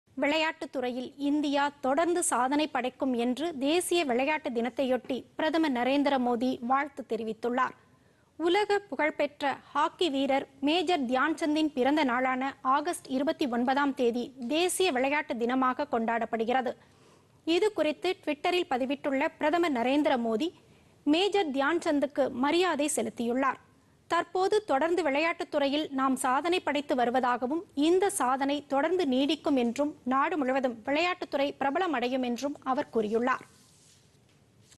விளையாட்டில் சாதனை படைப்போம் - பிரதமர்PodhigaiTamilNews
விளையாட்டுத்துறையில் இந்தியா தொடர்ந்து சாதனை படைக்கும் -  தேசிய விளையாட்டு தினத்தையொட்டி  பிரதமர் நரேந்திர மோதி வாழ்த்து.